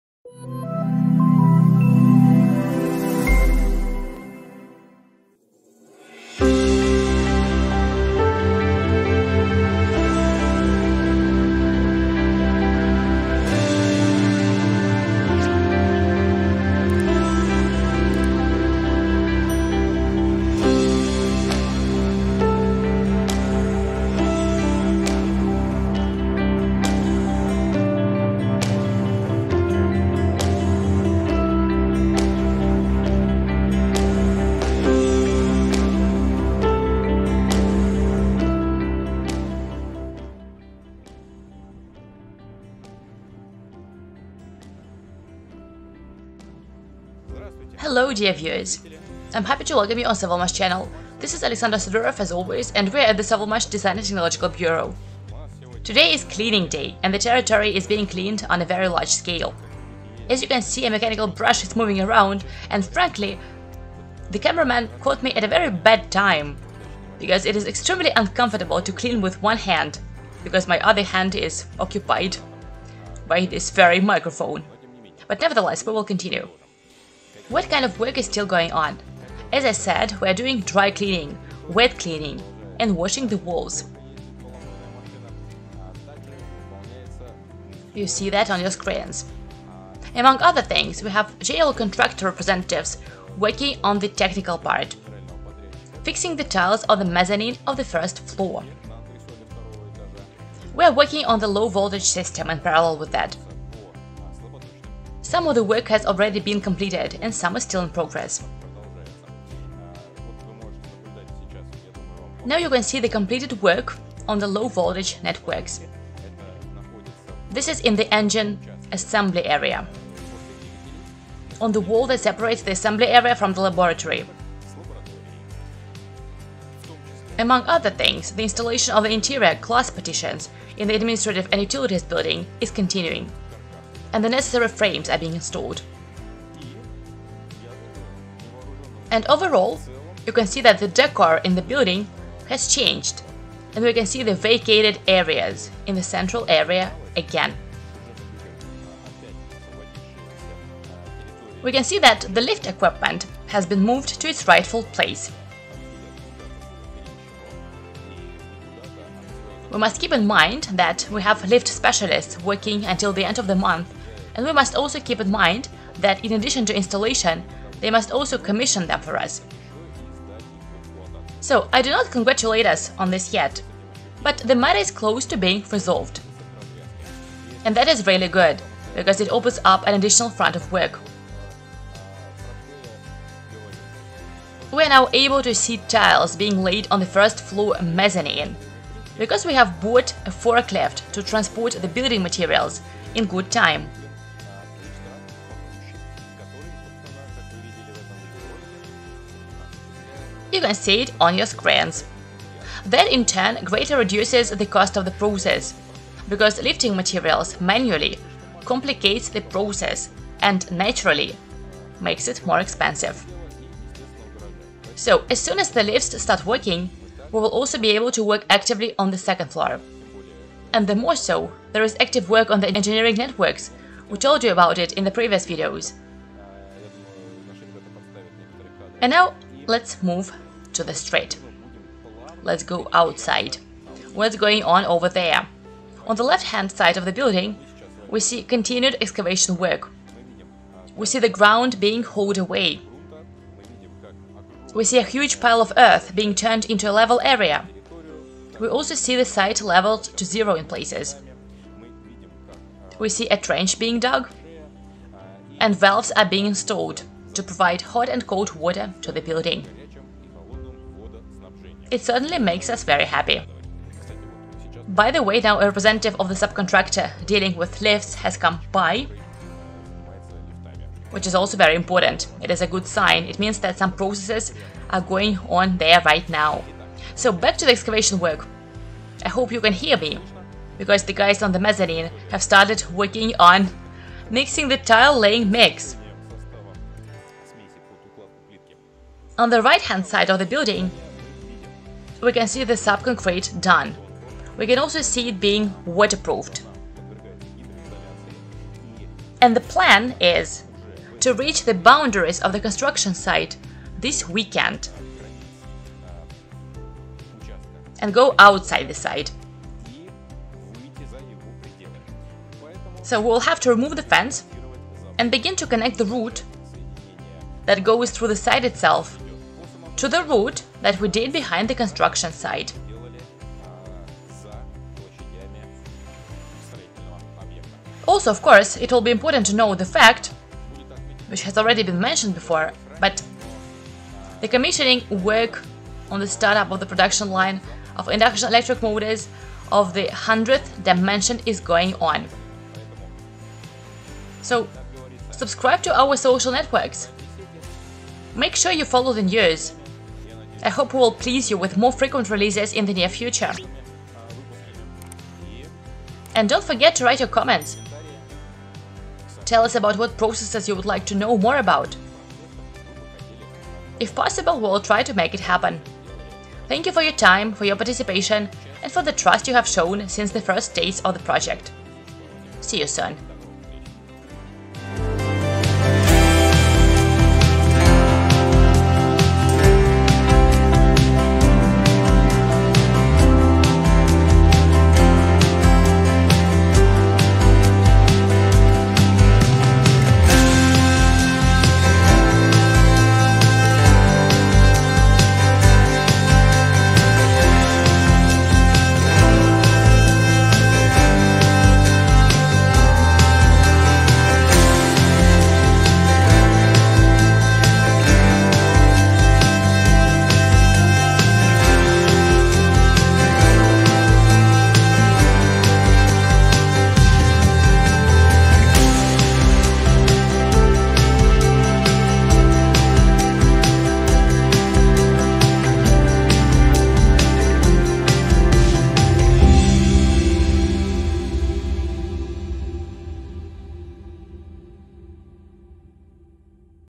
Clean-up day at the "Sovelmash" D&E, tiling, installation of elevators and other activities
Today, the D&E building is being wet and dry cleaned, including cleaning the walls.

Representatives of the general contractor are installing tiling on the second-floor mezzanine, while work is also being carried out on the low-current networks, and in the motor assembly area this work has already been completed.

The following activities are underway:

• installation of supports for the installation of transparent interior partition walls in the O&A;
• installation of elevators. Once the elevators are put into operation, an additional scope of work will open up:
• commissioning and start-up activities to launch the production line for size-100 induction electric motors;
• earthwork on the left hand side of the building: soil is being removed, the trench has been dug and the installation of plugs to provide the building with hot and cold water supply is underway.

Other work is in progress as well.

On the right hand side of the building, the concrete mattress has been completed and waterproofing is being done. The plan is to reach the boundaries of the construction site next weekend and go beyond it.

The work currently underway is quite extensive and requires everyone's attention for successful implementation of the project!

Each of you has the opportunity to make a contribution to the construction of the design and engineering technology department by purchasing new investment packages, upsizing and repaying your current packages early.

To find out how to make the most of it, check out the list of offers available now.